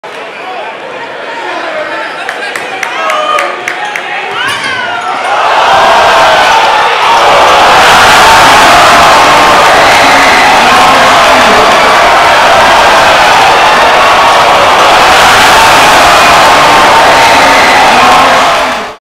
Travis Ingram DROPS defenders than soars MJ Style!!
Norcom's Travis Ingram drops defender than switches hands "MJ STYLE" at the Virginia Preps Classic. Norcom beats Cape Henry 64-49.